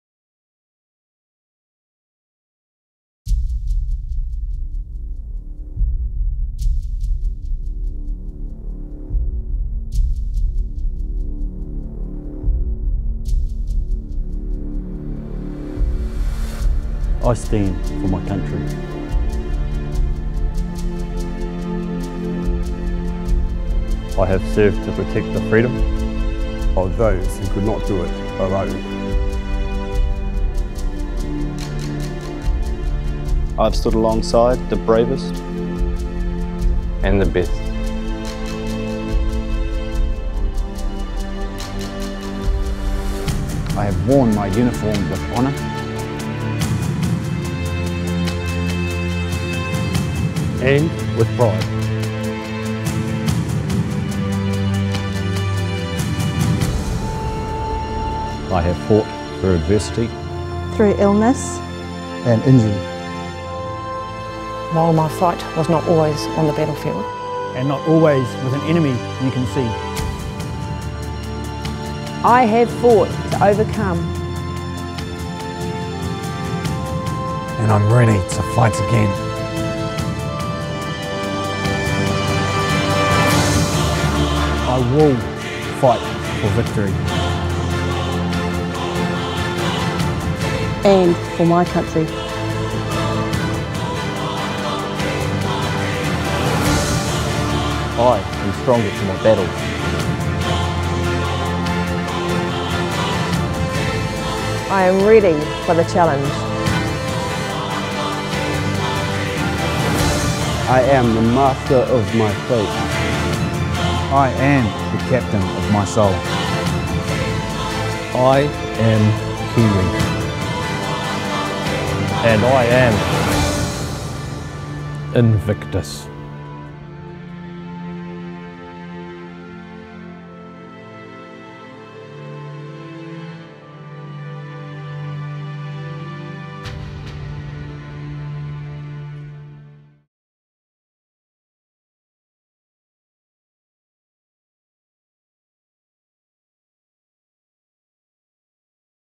INVICTUS GAMES 2016 - New Zealand Team Trailer
With sport at its core, The Invictus Games provide an international stage that unites wounded, ill, and injured Service men and women from around the world, both active duty and veteran, enabling them to learn and share through their common experiences.

The New Zealand Defence Force is proud to represent New Zealand at the 2016 Invictus Games Orlando 2016. We have 21 NZDF competitors from all Services, both current and ex-serving, getting
ready to take the fight to the ESPN Wide World of Sport Complex in Orlando, Florida from 8-12 May.

We’ll face off against over 500 competitors from 14 other countries in the spirit of comradeship, in a bid to bring back gold.

The commitment of those participating is testament to their individual courage, and to their conviction to face their challenges with a fighting spirit.